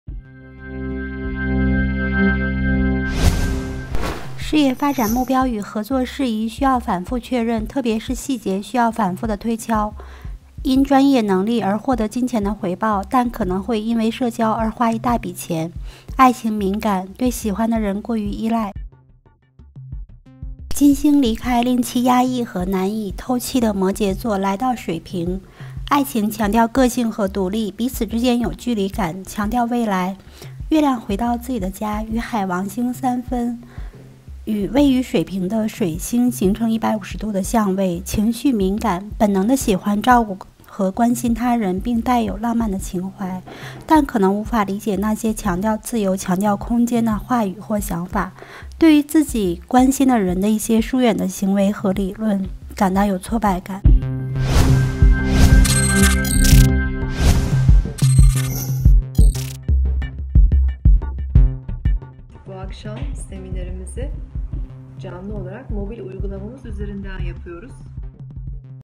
2月18日双鱼座
事业与合作反复确认，爱情敏感。